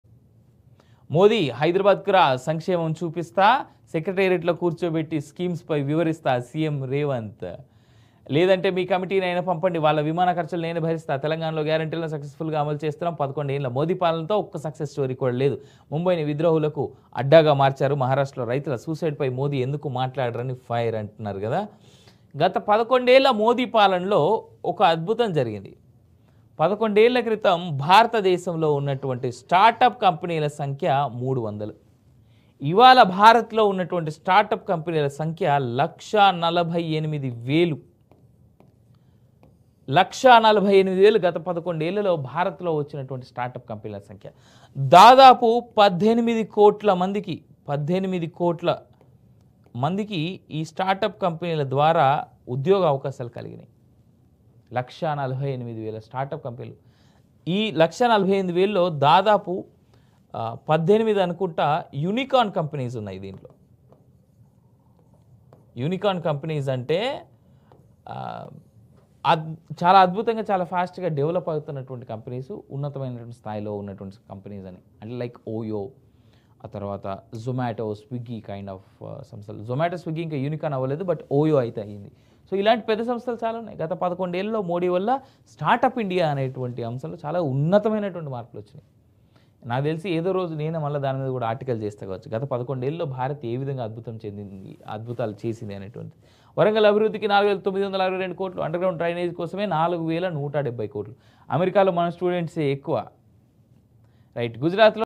మోదీ..హైదరాబాద్⁬​కు రా..సంక్షేమం చూపిస్త | Morning News With Avinash Janagama | BRK News
News Paper Analysis | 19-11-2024 | The First Edition | Morning News With Avinash Janagama | Telugu News Paper | @brknews.